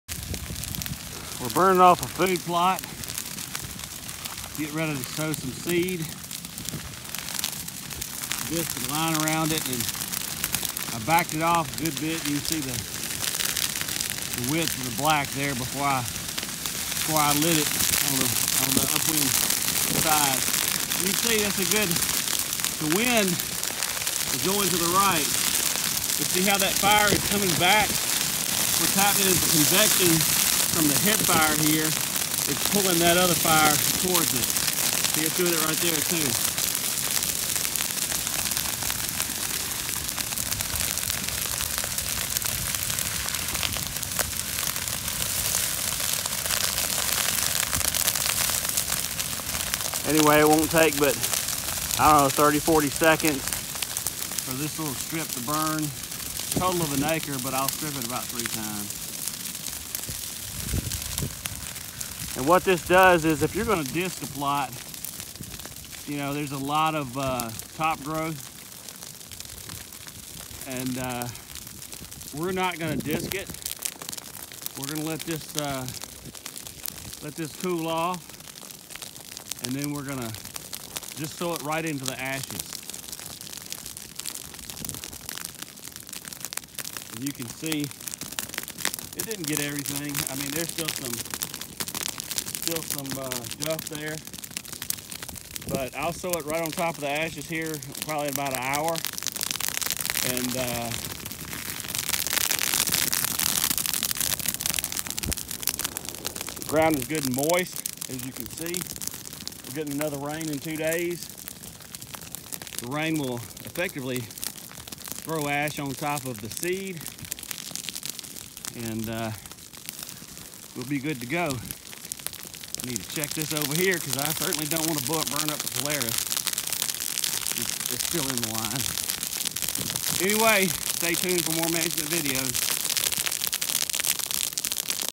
Burning off Food Plot
Prescribed fire can be an alternative to disking a wildlife opening.  Often fire is a better option if you don't plan to plant.  Under some circumstances, maintaining natural vegetation is better than planting anything.  Always be sure to follow all laws and have an experienced burner on site before you burn.